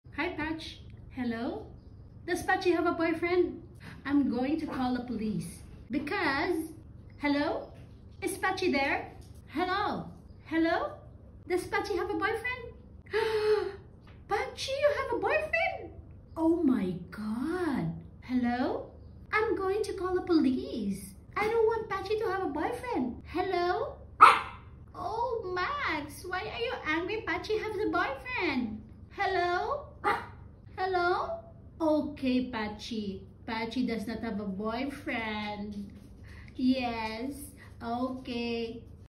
How my dog reacts to telephone call ☎️📞🐶♥️
Patchi's reaction to telephone call
No boyfriend until age 9!